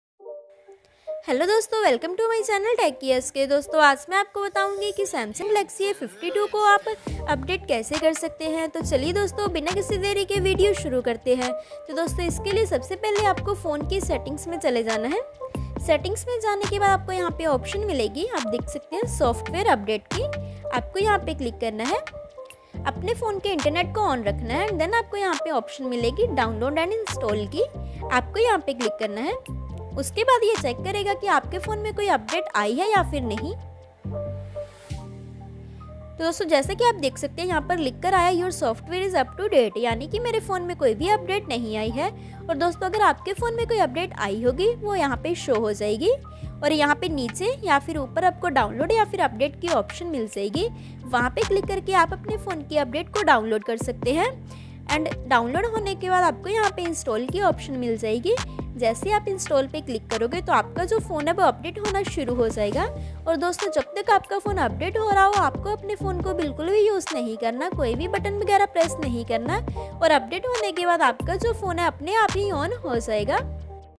how to update Samsung Galaxy A52 || Samsung Galaxy A52 ko update kaise kare
Hello dosto, aaj maine is video me bataya hai ki Samsung Galaxy A52 ko update kaise kare, dosto Samsung Galaxy A52 ko update karna bahut hi aasan hai. Agar aap bhi janna chahate hain ki Samsung Galaxy A52 ko update kaise kare to is video ko pura dekhiyega, thanks friends.